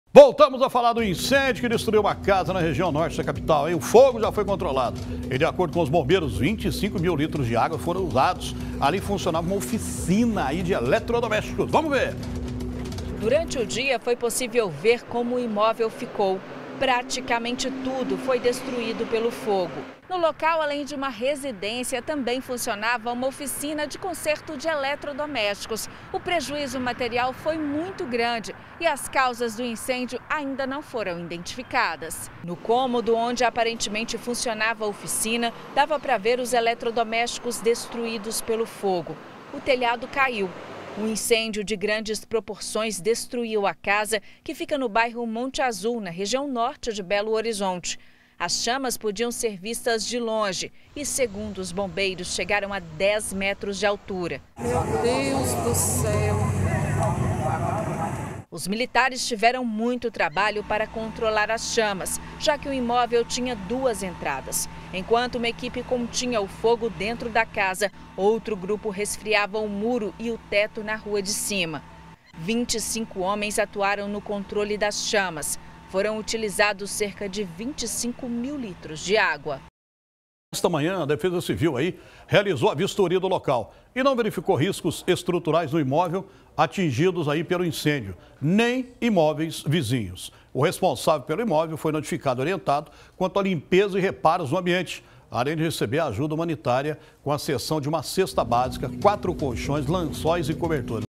Incêndio destrói casa e oficina de eletrodomésticos em BH
Um incêndio destruiu uma casa no bairro Monte Azul, na região Norte de Belo Horizonte. No local, além de uma residência, funcionava uma oficina de conserto pde eletrodomésticos. De acordo com o Corpo de Bombeiros, 25 mil litros de água foram usados no local.